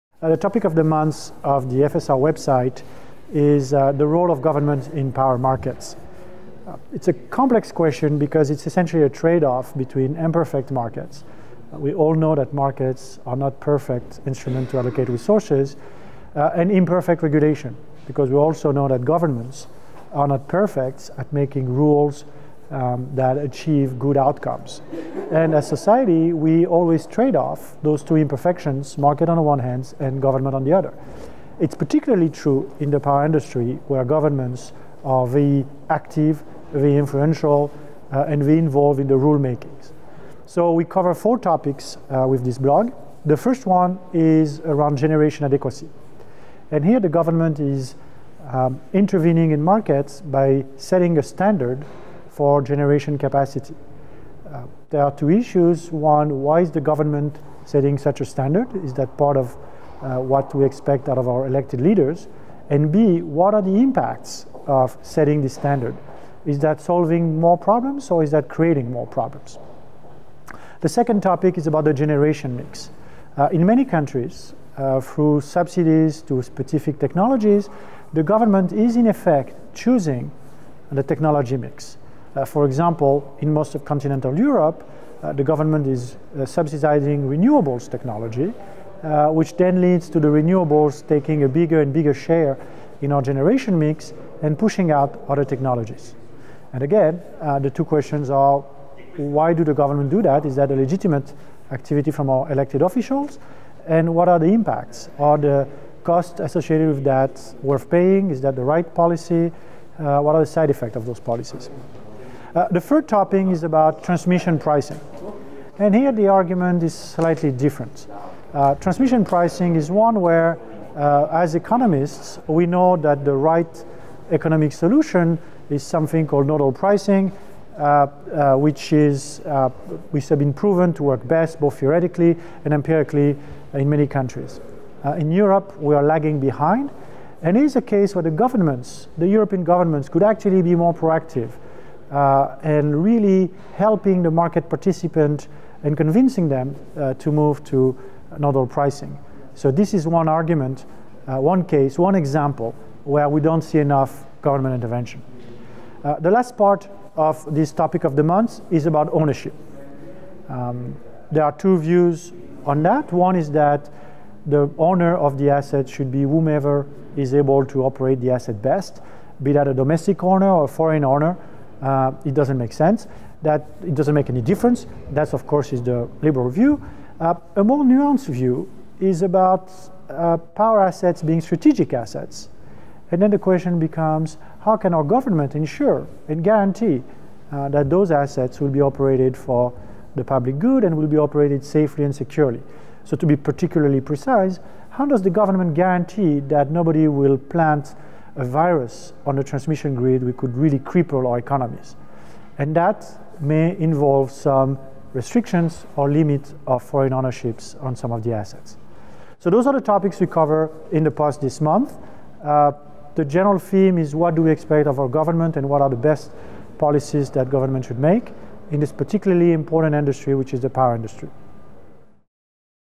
Thomas-Olivier Léautier | Role of governments in the electric power industry
December 2016 FSR topic of the month explores governments’ intervention in four aspects of the power industry: (i) generation adequacy, (ii) generation mix, (iii) organization of the transmission grid, and (iv) asset ownership. This week’s post discusses the role of governments in shaping the generation mix.

In planned economies, governments choose the power production technologies.  Civil servants examine the different technologies available at a given time, compare their costs, and determine the optimal mix.  In the vertically integrated and regulated monopolies of old, utility planners were performing this task, in cooperation with civil servants.